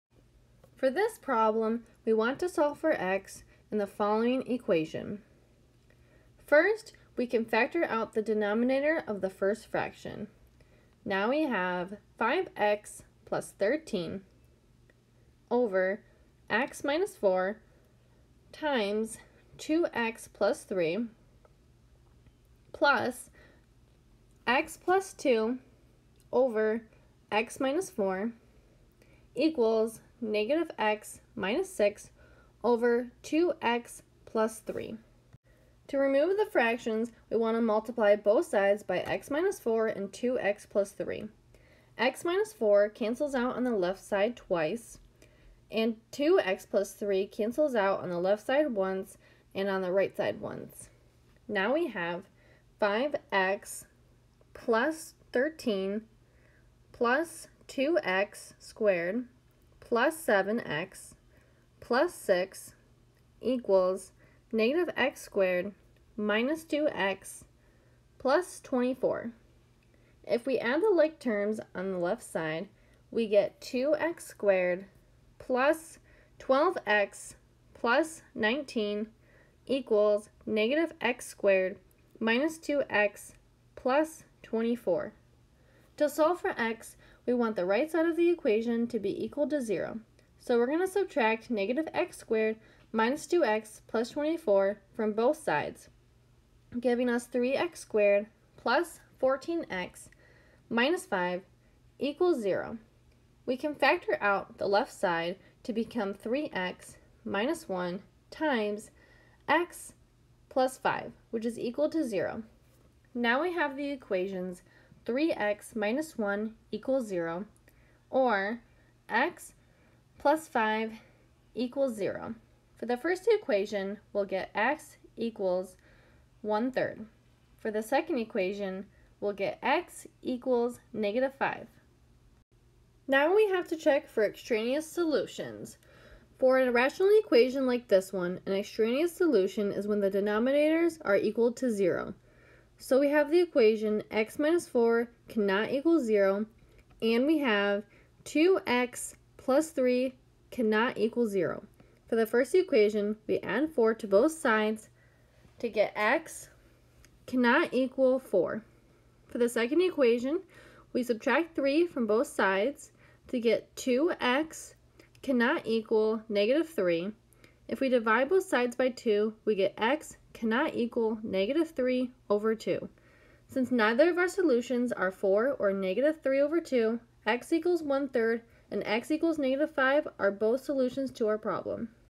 Rational Equation: Solve (5x+13)/(2x^2-5x-12) + (x+2)/(x-4) = (-x-6)/(2x+3)
Erin from SVSU Micro Math helps you solve a rational equation.

Problem: Solve (5x+13)/(2x^2-5x-12) + (x+2)/(x-4) = (-x-6)/(2x+3)